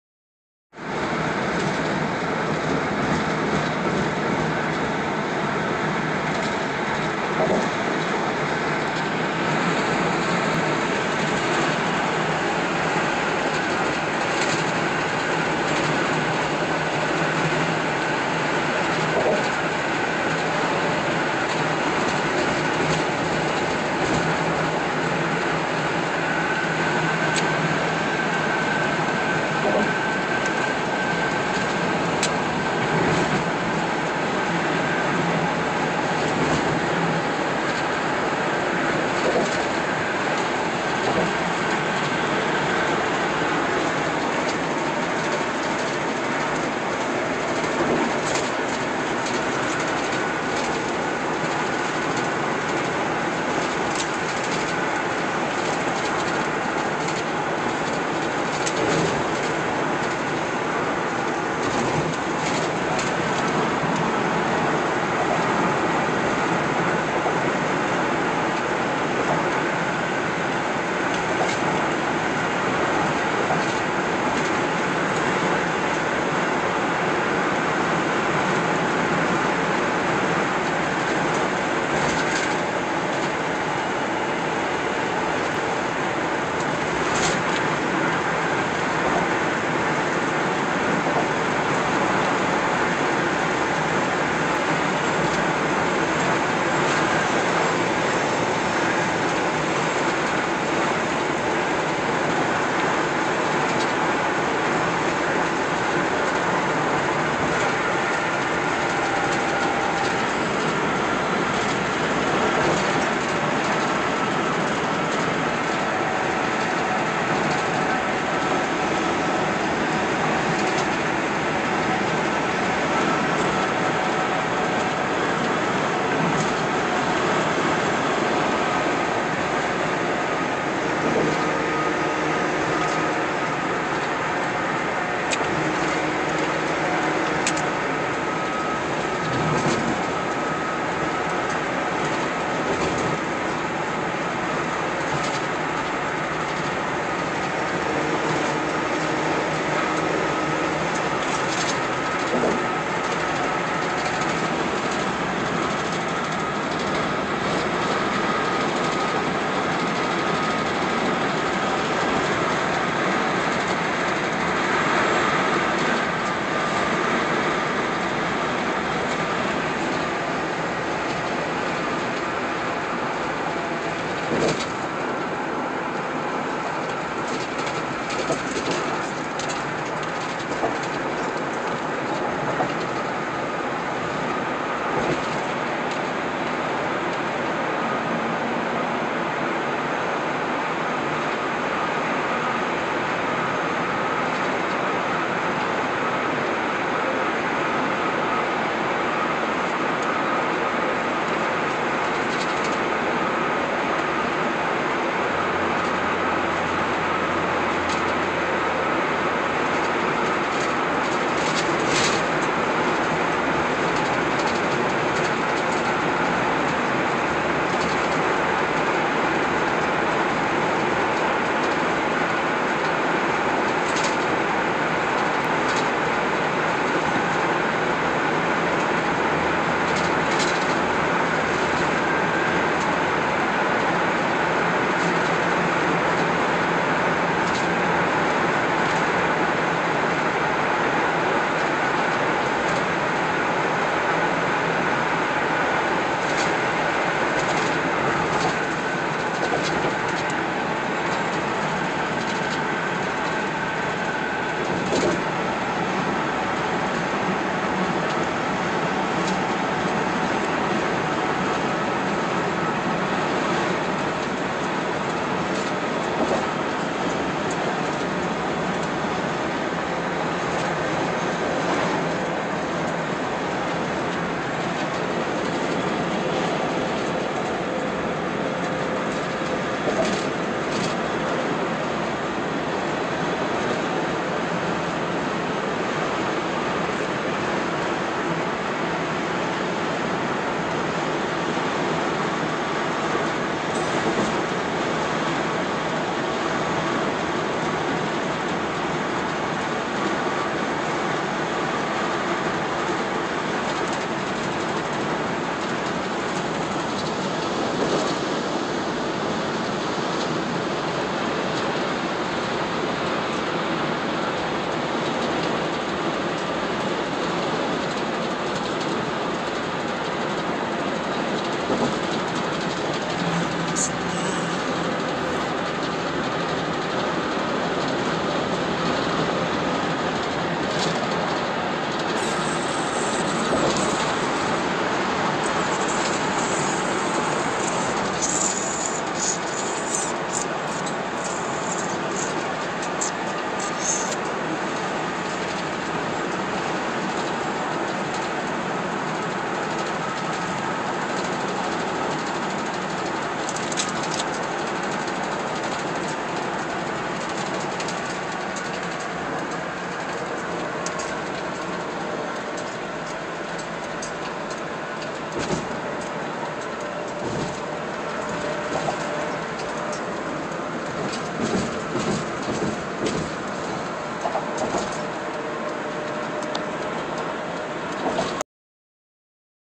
H3.3GP
Going thru the tunnel on H3 with beautiful mountains...